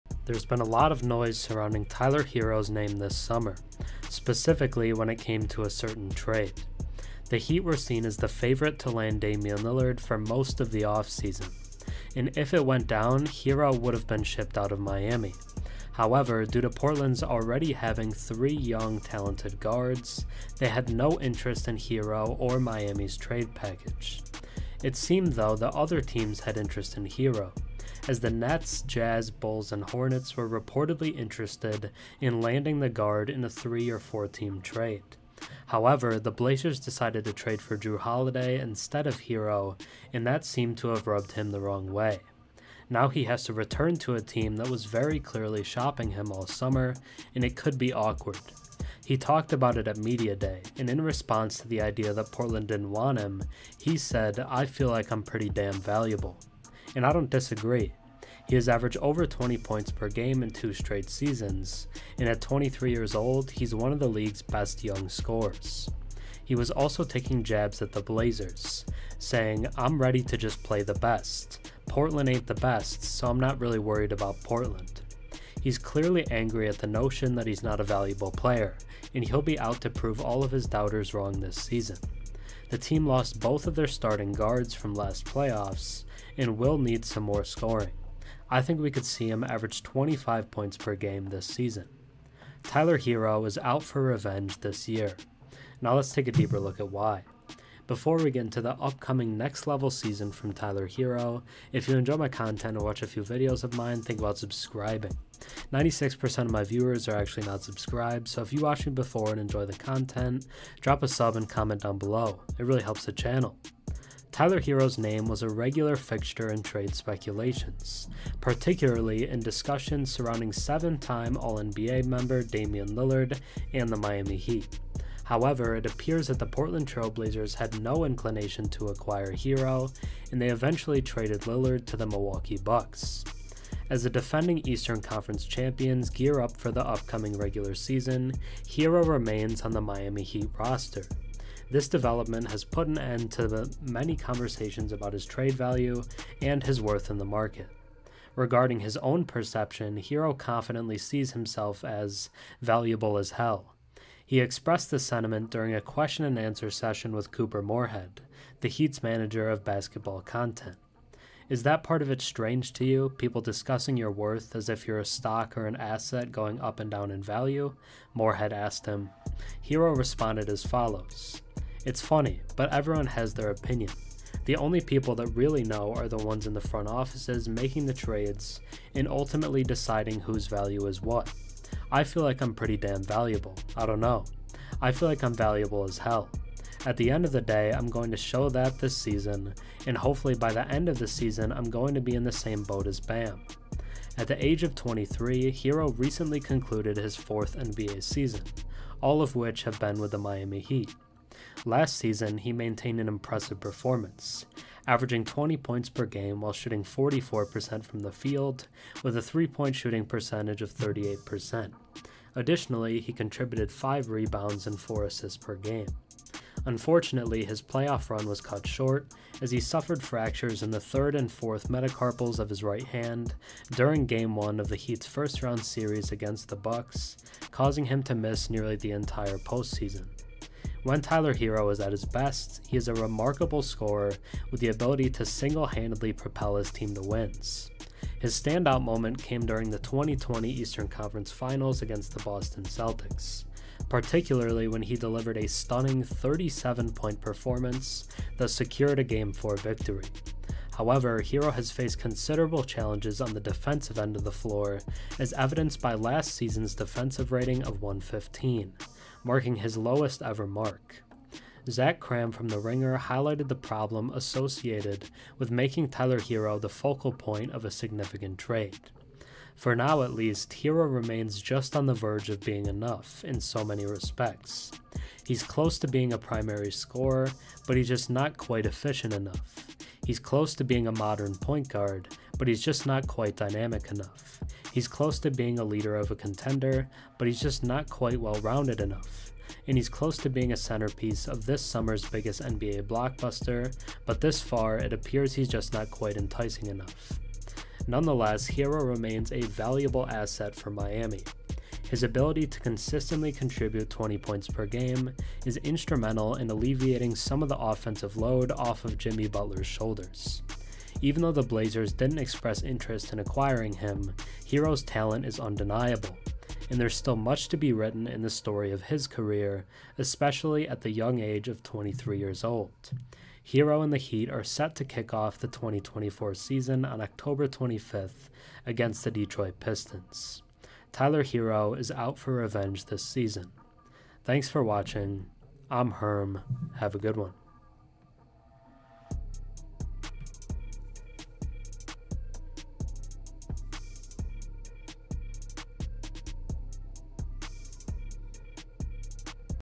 Tyler Herro Is Out For Revenge This Season...
There’s been a lot of noise surrounding Tyler Herro’s name this summer, specifically when it came to a certain trade. The Heat were seen as the favorite to land Damian Lillard for most of the offseason, and if it went down Herro would’ve been shipped out of Miami. However, due to Portland already having three young, talented guards they had no interest in Herro or Miami’s trade package. It seemed though that other teams had interest in Herro, as the Nets, Jazz, Bulls, and Hornets were reportedly interested in landing the guard in a three or four team trade. However, the Blazers decided to trade for Jrue Holiday instead of Herro, and that seemed to have rubbed him the wrong way. Now he has to return to a team that was very clearly shopping him all summer, and it could be awkward. He talked about it at media day, and in response to the idea that Portland didn’t want him, he said “I feel like I’m pretty damn valuable”. And I don’t disagree. He has averaged over 20 points per game in two straight seasons, and at 23 years old, he’s one of the league’s best young scorers. He was also taking jabs at the Blazers saying, “I’m ready to just play the best. Portland ain’t the best, so I’m not really worried about Portland.” He’s clearly angry at the notion that he’s not a valuable player, and he’ll be out to prove all of his doubters wrong this season. The team lost both of their starting guards from last playoffs, and will need some more scoring. I think we could see him average 25 points per game this season. Tyler Herro Is out for revenge this season.

Think about subbing, I'm on the road to 11k!!!